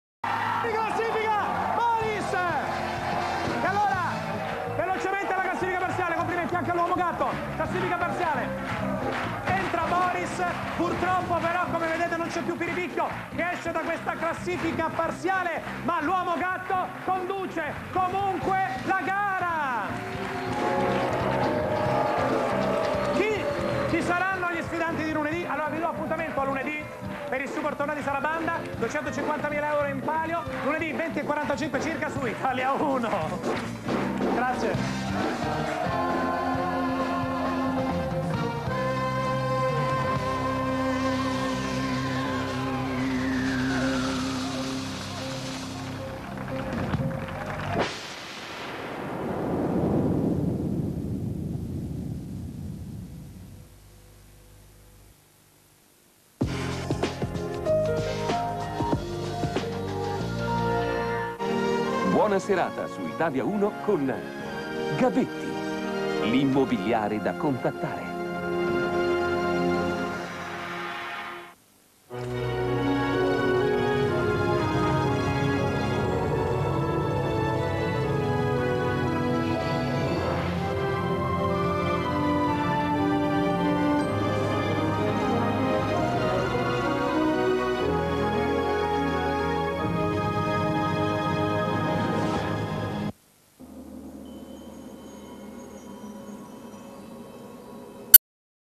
Sequenza Italia 1 - Venerdì 18 Marzo 2005 [1]
Sequenza Italia 1 - Venerdì 18 Marzo 2005 [1] trasmessa prima del film "Ultima Missione"